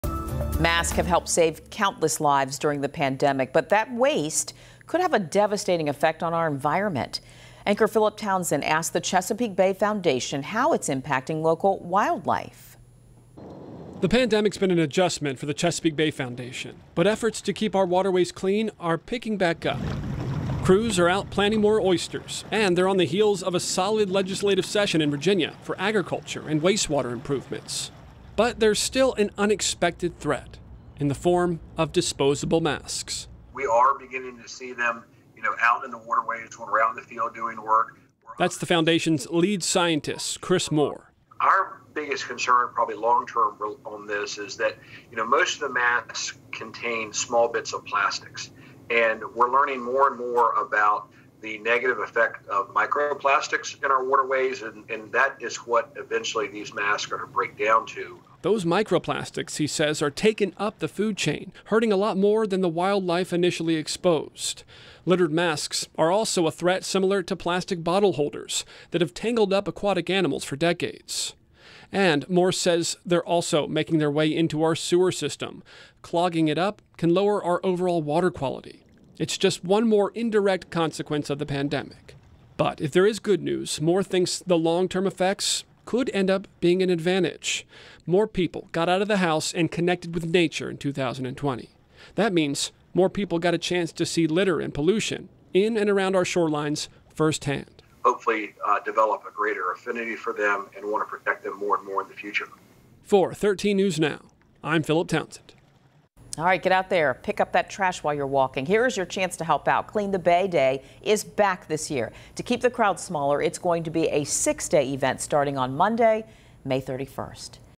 How masks are impacting local wildlife
Masks have helped save countless lives during the pandemic, but the waste they cause could have a devastating effect on our environment. Anchor Philip Townsend asked the Chesapeake Bay Foundation how discarded masks are impacting local wildlife.